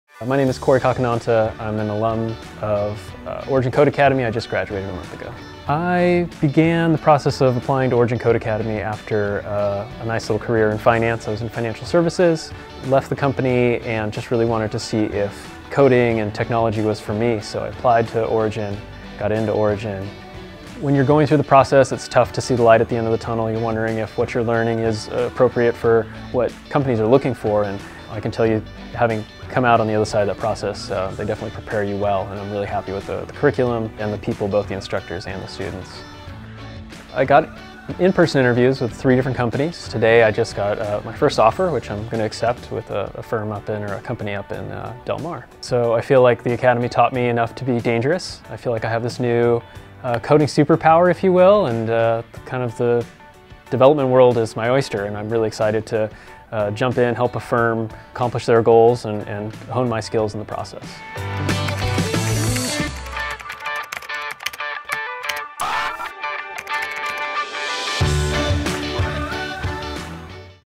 How an Origin Code Academy Graduate Developed His New Coding Superpower
Corey Cacanata is an alumni of Origin Code Academy. He came from a career in finance
and financial services. He applied to Origin to see if coding and technology was for him.

After graduating, he got his first job offer. For him, the development world is his oyster where he can help a firm accomplish their goals and hone his skills in the process.

➡️ Are you ready to learn how to code in 12 weeks? Schedule an in-person tour now

➡️ Join Our Software Developer Bootcamp. Classes start every month

Well, my name is Corey Cacanata. I'm an alumni of Origin Code Academy. I just graduated a month ago.

I began the process of applying to Origin Code Academy after a nice little career in finances
and financial services, left the company and just really wanted to see if coding and
technology was for me.

So I applied to Origin, got into Origin. When you're going through the process, it's tough to see the light at the end of the tunnel, and you're wondering if what you're learning is appropriate for what companies are looking for. And I can tell you, having come out on the other side of the process, they definitely prepare you well, and I'm really happy with the curriculum and the people, both the instructors and the students.

I got in person interviews with three different companies. Today I just got my first offer, which I'm going to accept with a firm up in, or a company up in Del Mar. So, I feel like the academy taught me enough to be dangerous. I feel like I have this new coding superpower if you will, and, kind of, the development world is my oyster, and I'm really excited to jump in, help a firm accomplish their goals and hone my skills in the process.